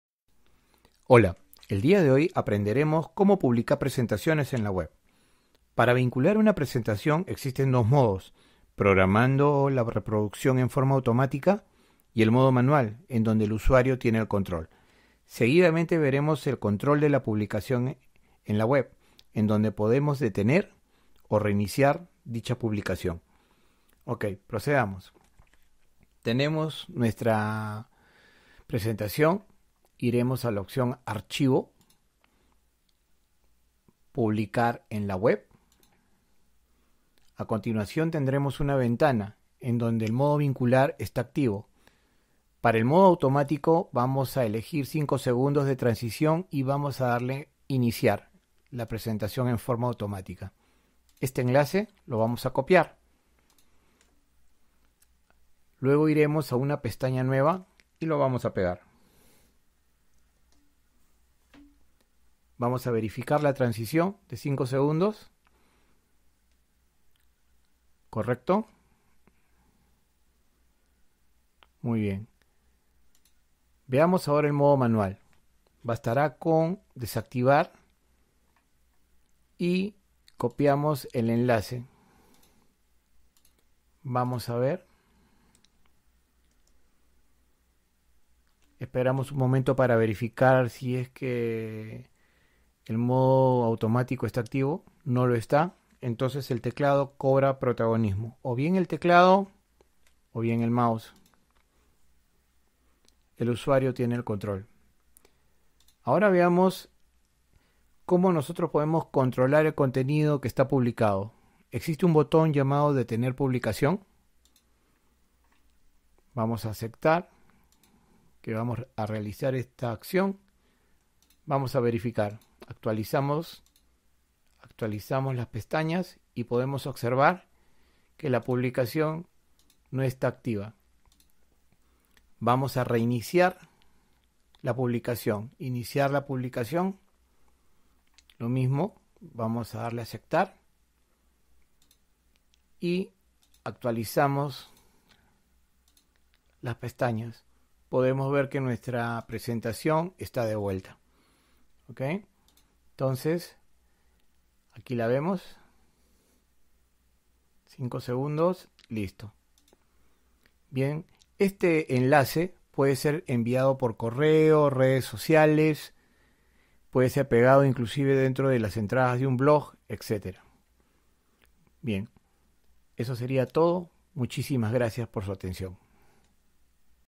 ¿ Cómo publicar presentación ( Google Slide) en la web ?
En esta sesión aprenderemos cómo publicar nuestras presentaciones en la web. Para ello vamos identificar las dos formas de hacerlo: automática y manual. Luego, veremos el control de la publicación a través de la detención o inicio de la publicación.